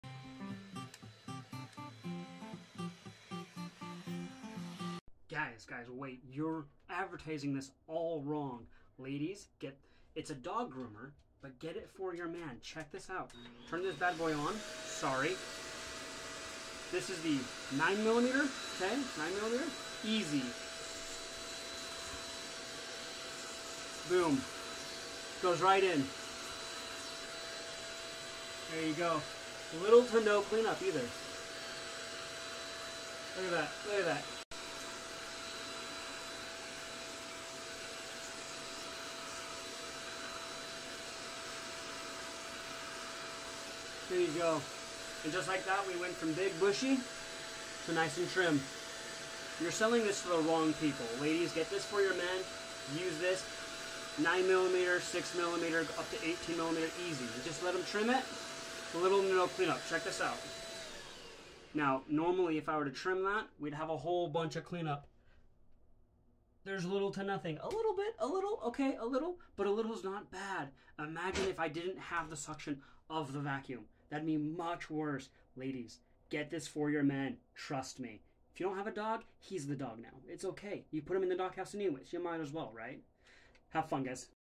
you're advertising this all wrong.
This is a dog groomer that doubles as a beard trimmer.
Ladies get this for your man!
Guys get this so your bathroom isn't covered in hair when she comes over.